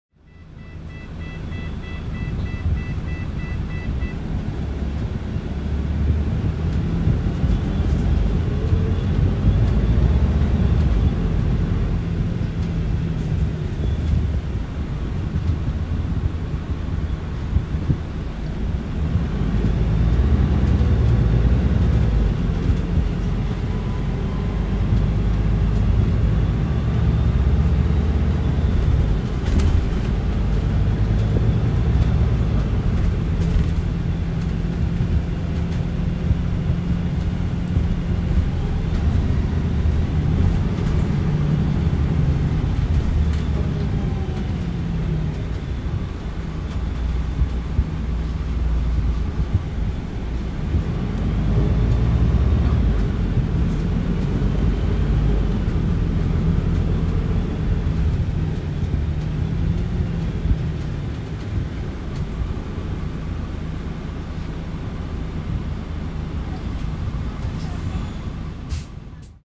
[Bus Audio] PITCL HC1387@NR330 (MAN RR8 | ZF Ecolife II)
Bus Information:
Engine: MAN D1556LOH-16 (Euro 6d, 360hp)
Gearbox: ZF Ecolife 2 6AP1720B
Date of Registration: 2024/04/30
Bodywork: Gemilang Lion's Intercity
VIN: WMARR8ZZXPF021395

Engine Braking? Disabled!